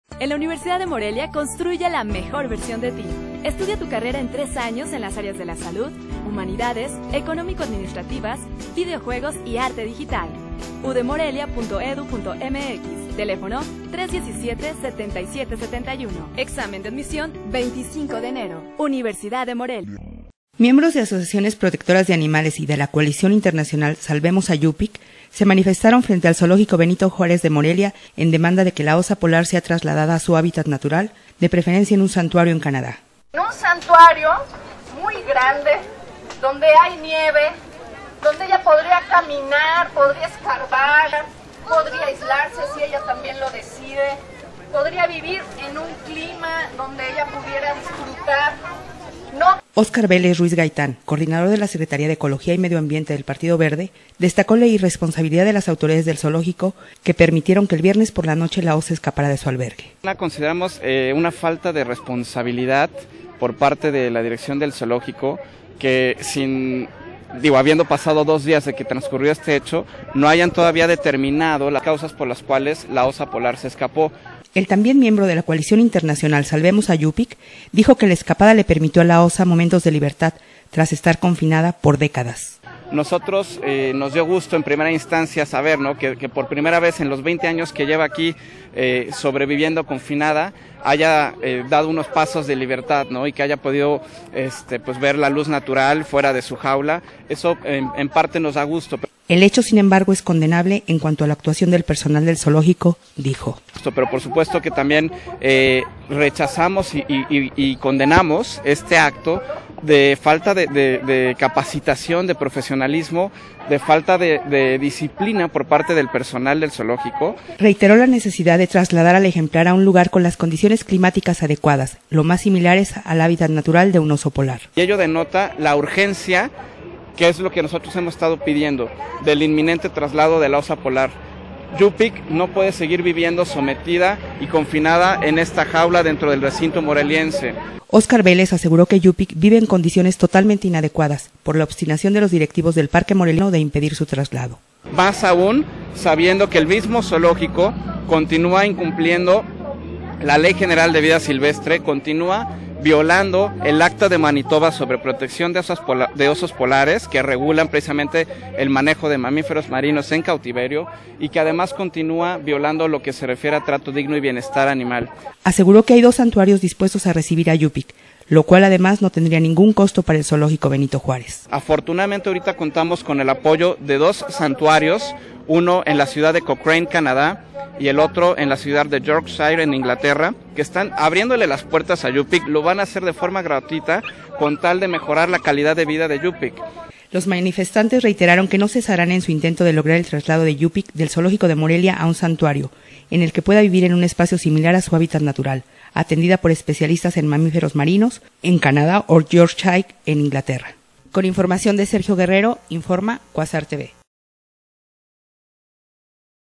Demandan trasladar a osa Yupik a su hábitat natural
Miembros de asociaciones protectoras de animales y de la Coalición Internacional "Salvemos a Yupik", se manifestaron frente al Zoológico Benito Juárez de Morelia, en demanda de que la osa polar sea trasladada a su hábitat natural, de preferencia a un santuario en Canadá...